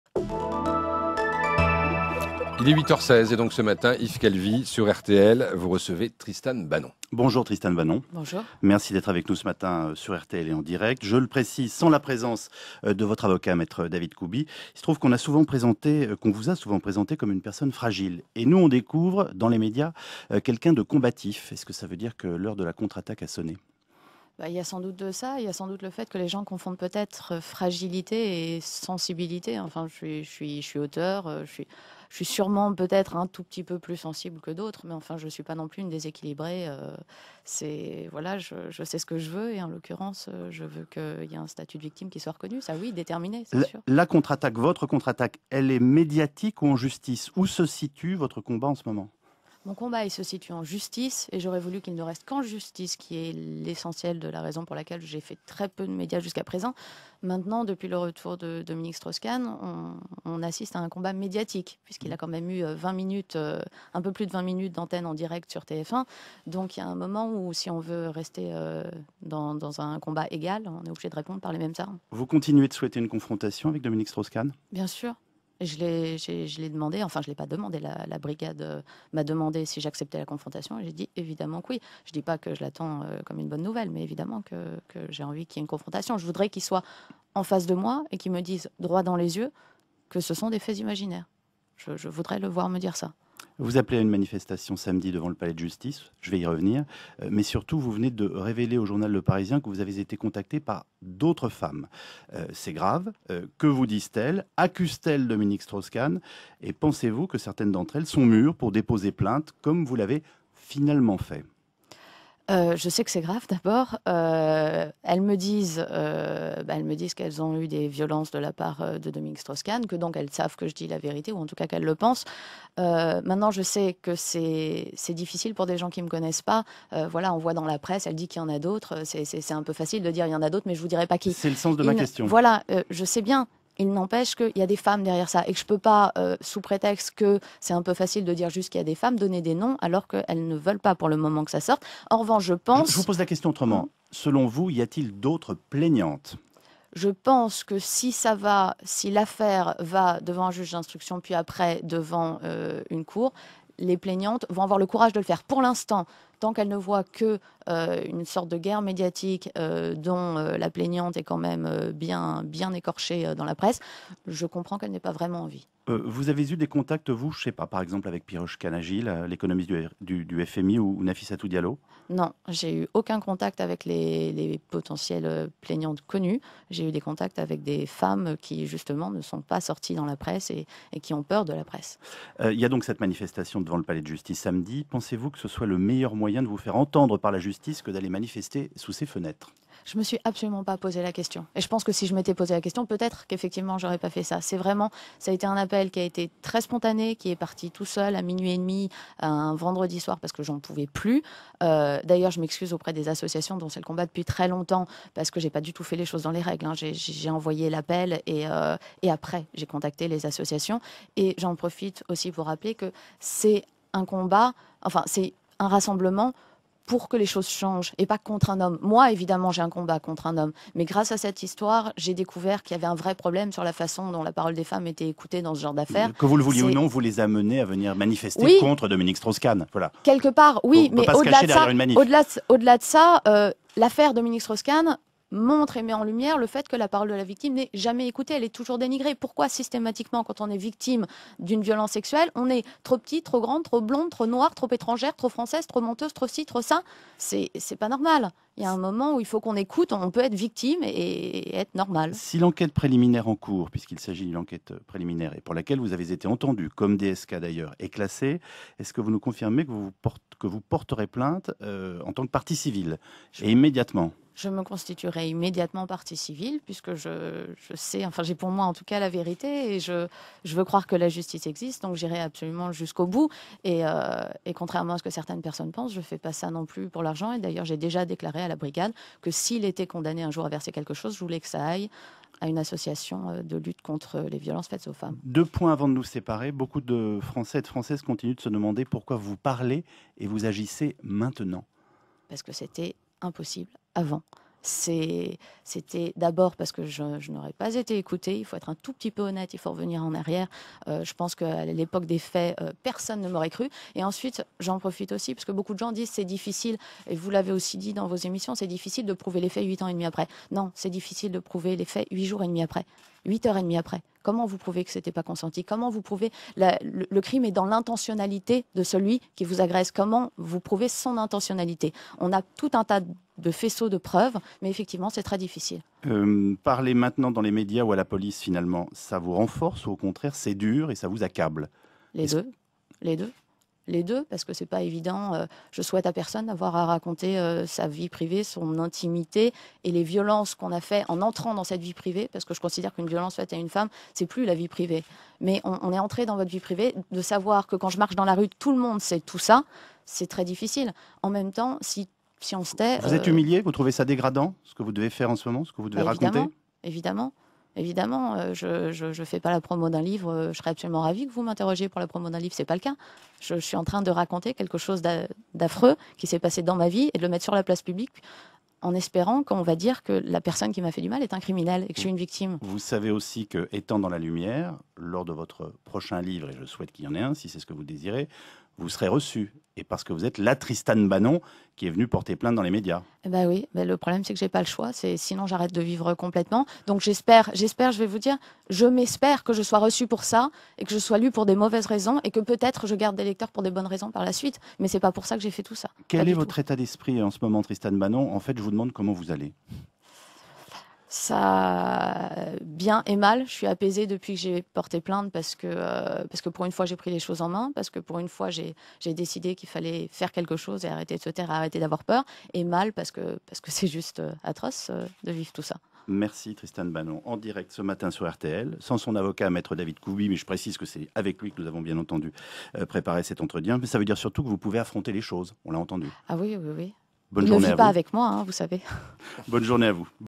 La romancière et journaliste Tristane Banon sur RTL : J'irai jusqu'au bout ! - RTL - RTL
La romancière et journaliste Tristane Banon sur RTL : J'irai jusqu'au bout !